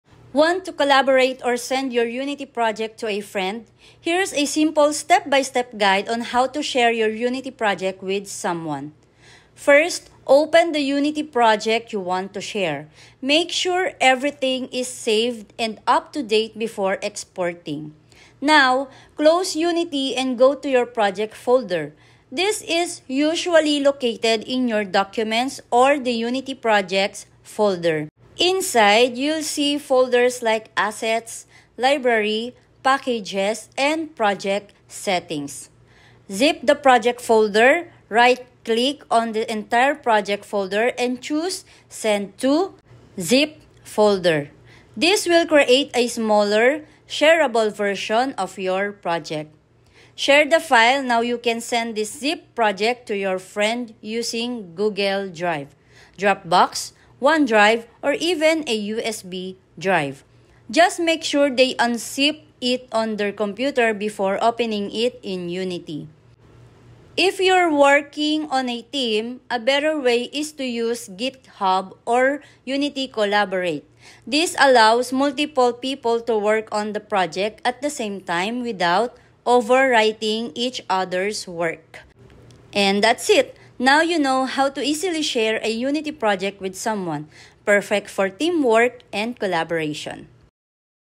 HOW TO SHARE UNITY PROJECT WITH SOMEONE 2026! (FULL GUIDE)
In this video I am going to show you how to share a Unity project with someone easily. Learn how to send Unity projects, collaborate with others, and share files using GitHub or Unity Collaborate.